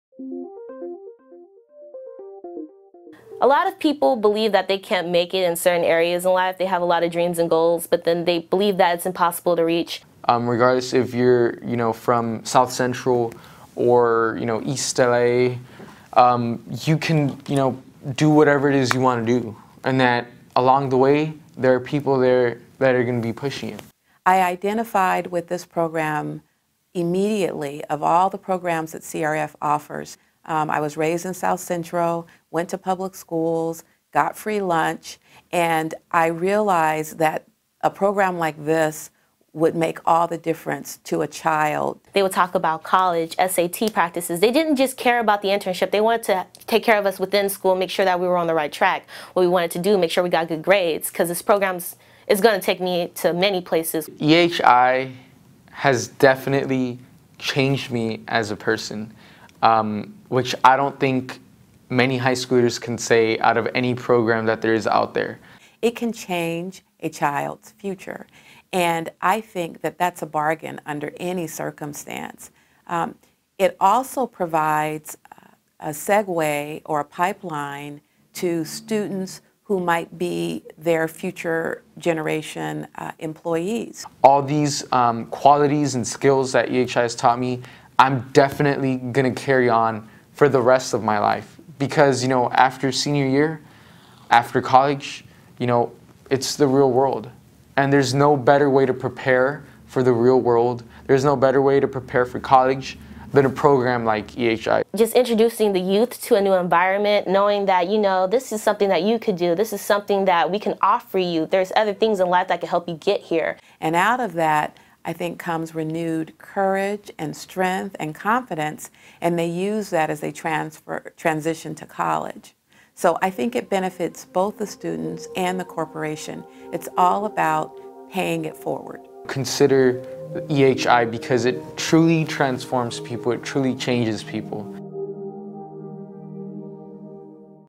Expanding Horizons Internships - Constitutional Rights Foundation
Constitutional Rights Foundations,s Expanding Horizons Internships (EHI) combine paid internships in exciting professional settings with 20 interactive seminars. The program starts in the spring and concludes in the summer with the paid summer internship. EHI provides professional skill-building, community awareness, and educational and college preparation seminars necessary for effective action in our democratic society.